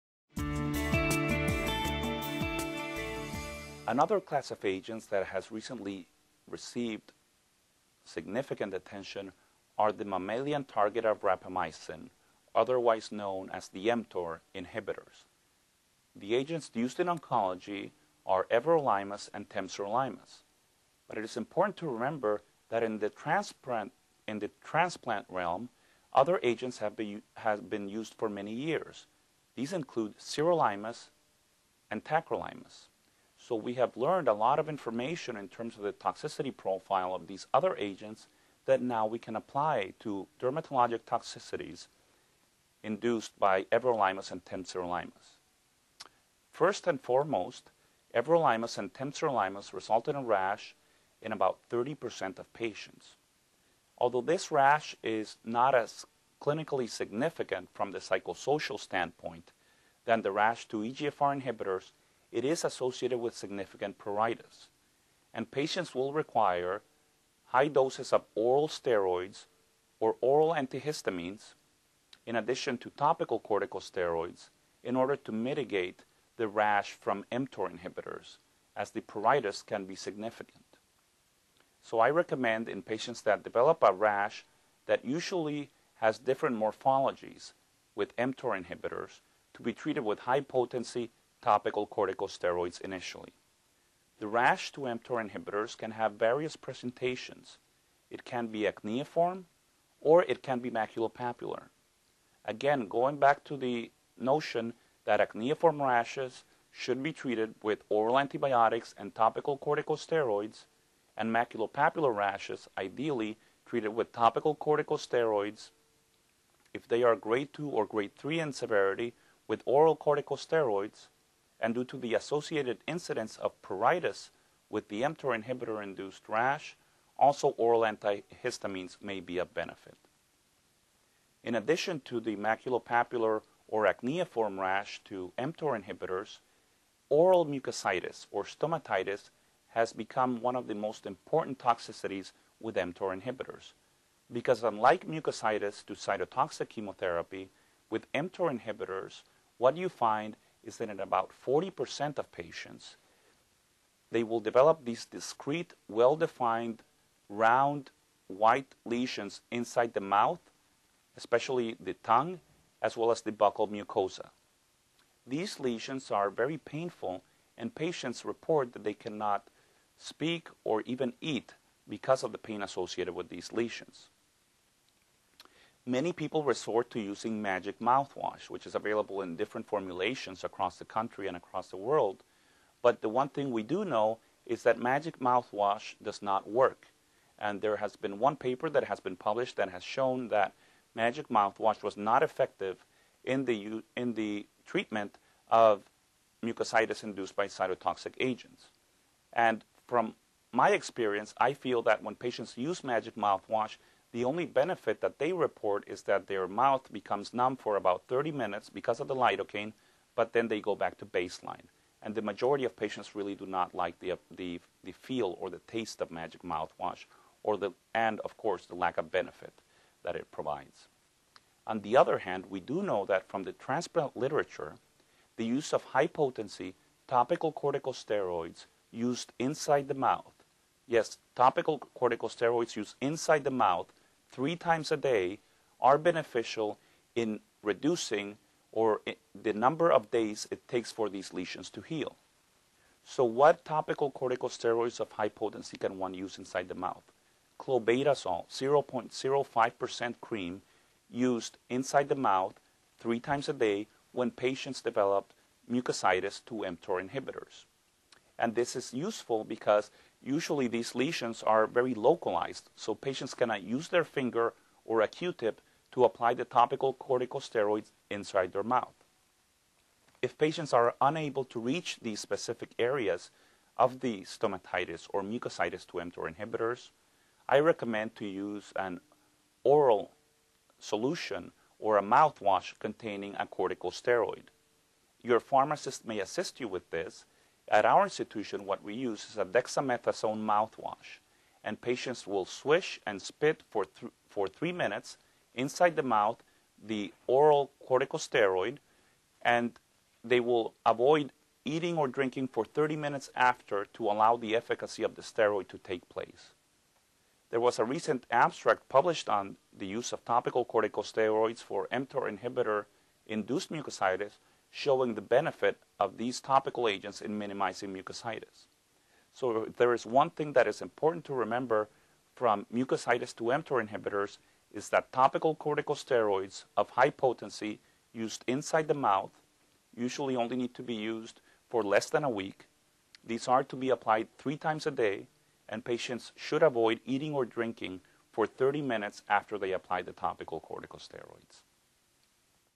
Managing mTOR Inhibitor Induced Adverse Events
Mario E. Lacouture, MD, a dermatologist at the Memorial Sloan-Kettering Cancer Center in New York City, discusses strategies for managing mTOR inhibitor-associated dermatologic toxicities.